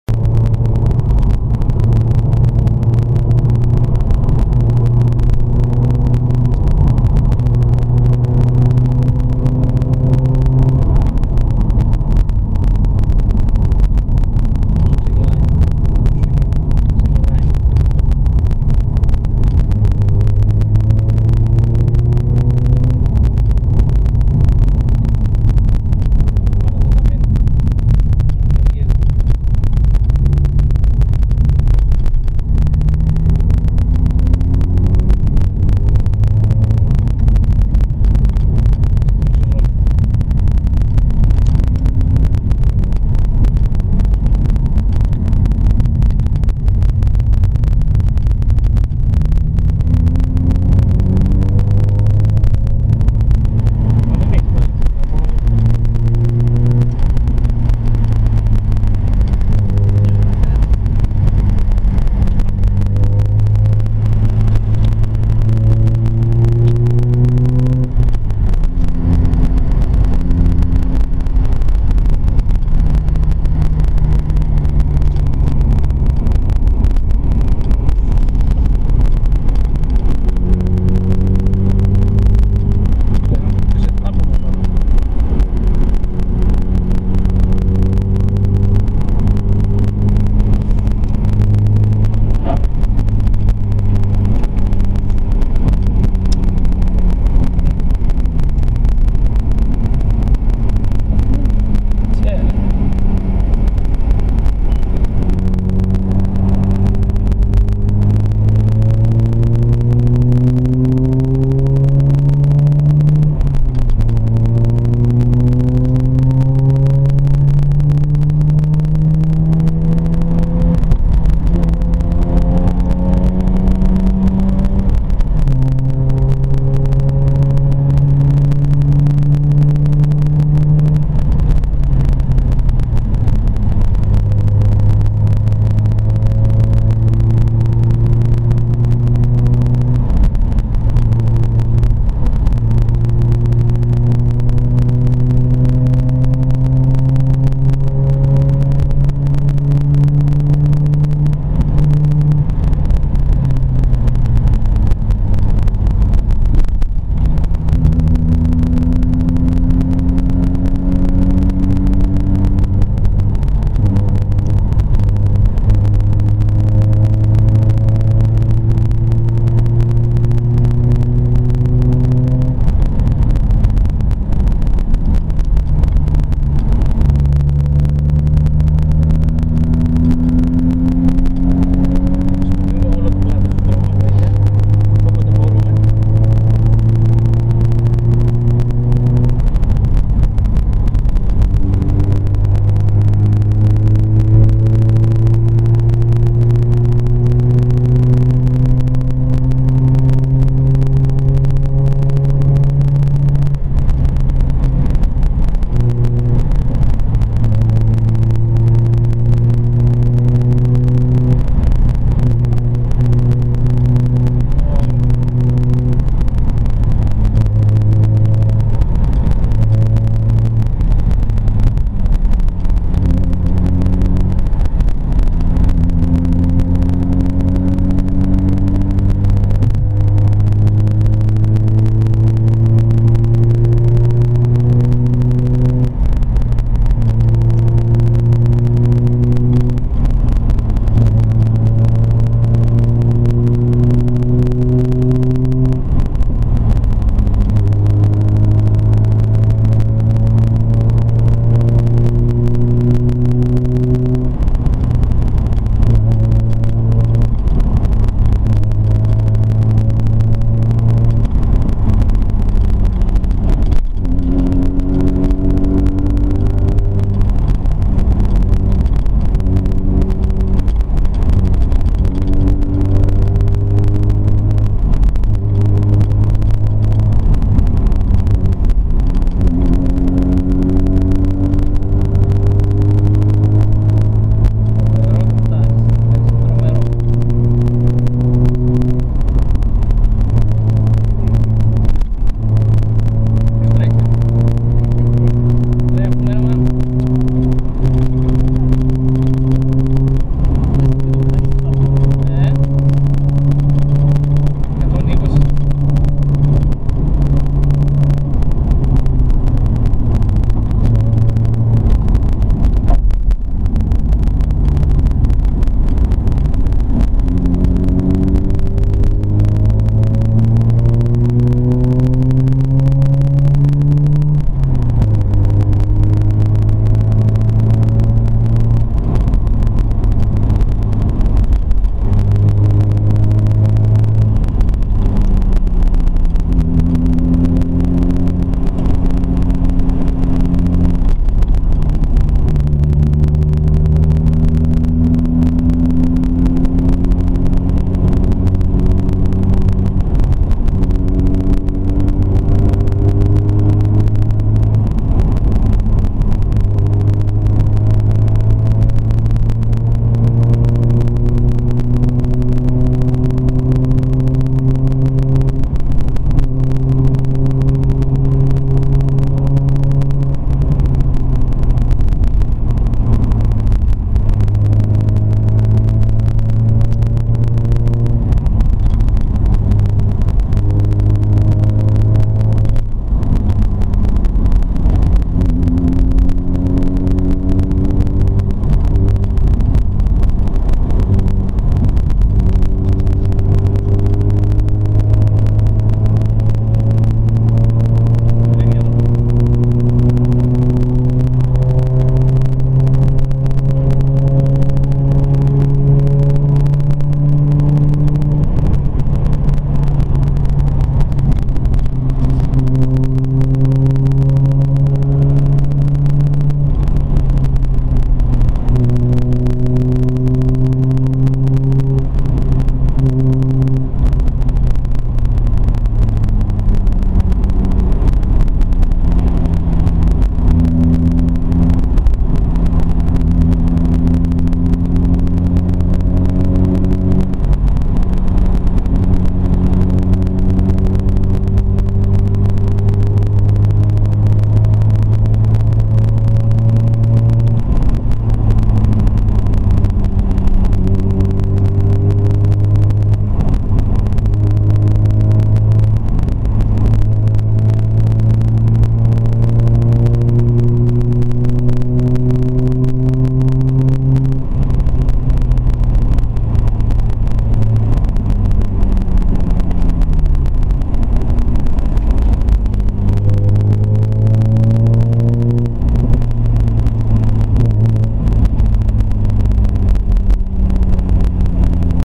rx8 from inside.
ληψη απο μεσα,σε διαδρομη απο αρνισσα-αγιος παντελεημονας,μεσω της πανεμορφης λιμνης βεγοριτιδας,με λιγη βροχη και ολα τα ηλεκτρονικα βοηθηματα του οχταριου off.